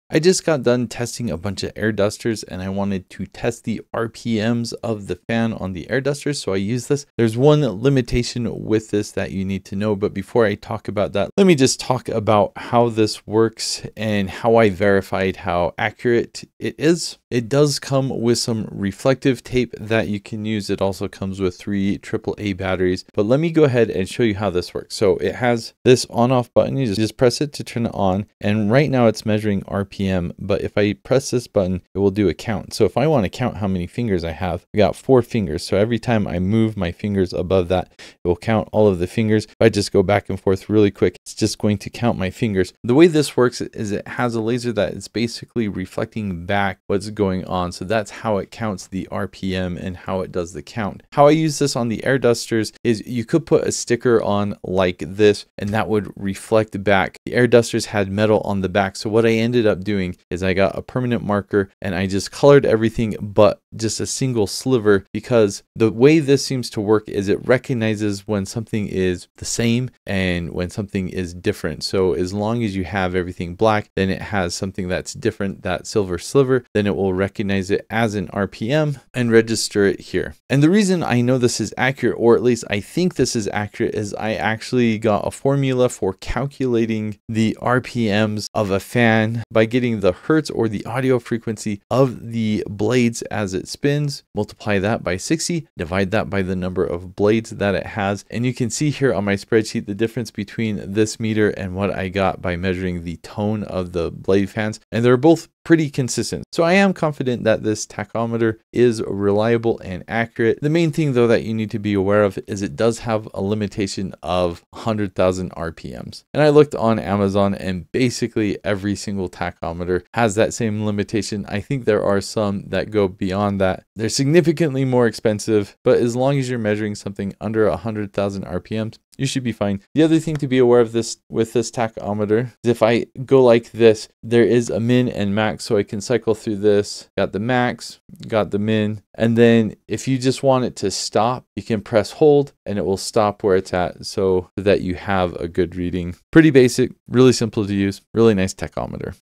Inexpensive Tachometer - What you should know for best results. UNI-T UT373 Mini Digital Tachometer
Info listed on the Amazon product page:

UNI-T UT373 Mini Digital Tachometer, 10~99999 RPM Non-Contact Tachometer Speed Meter Speed Gauge with Backlight, Max/Min/Data Hold, Rotating Speed/Count Function Switching

Product details
Top highlights
【NON-CONTACT TACHOMETER】The digital tachometer is able to safely measure rotational objects through a non-intrusive method, and it can solve measuring problems of some tested objects which is easy to deform.It is used to measure rotation speed of motors and other machines.
【MULTIFUNCTION】This non-contact tachometer can RPM measurement and revolution count. The measuring range of RPM is 10~99999 rpm, and the counting measurement is 0 ~ 99999 counts. It also have the maximum/minimum measurement mode, data retention mode, overrange display, low battery indication, auto power off and other functions.
【LCD BACKLIGHT AND COMPACT】The LCD backlight design of this speed meter allows you to read clearly and effectively even in harsh environment or dark environment. The fuselage is small, and the shape of the fuselage has been redesigned, which is better controlled in the hand and does not damage the appearance.
【SAFE AND RELIABLE】UT373 speed gauge is a digital non-contact tachometer with stable performance, safety and reliability. The circuit design of the whole machine takes the microcontroller as the core, and the photoelectric speed sampling processing makes it a tachometer with superior performance.
【CUSTOMER SERVICE】We provide 6-month worry-free warranty and friendly customer service.